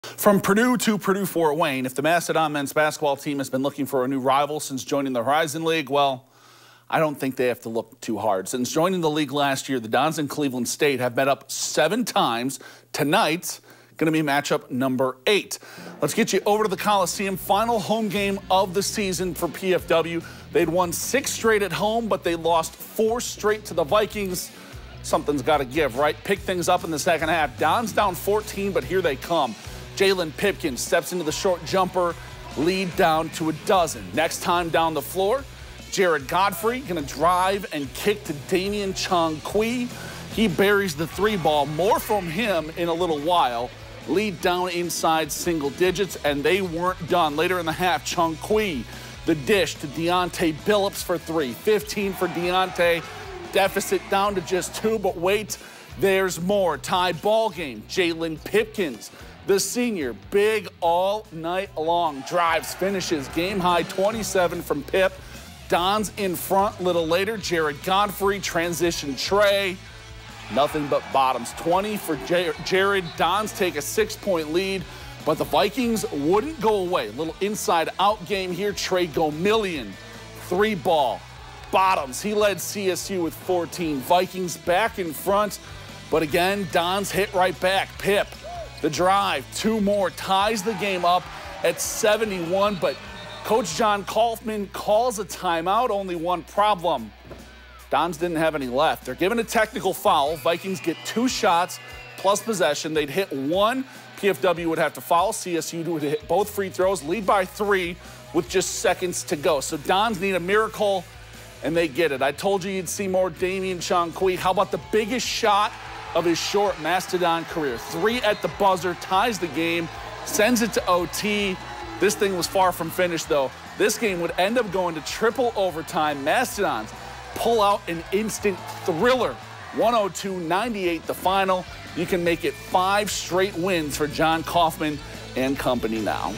Mastodons close home slate with thriller over Cleveland State
The Purdue Fort Wayne men's basketball team knocked off Cleveland State 102-98 in triple overtime to close the home portion of their 2021-22 schedule on Monday night.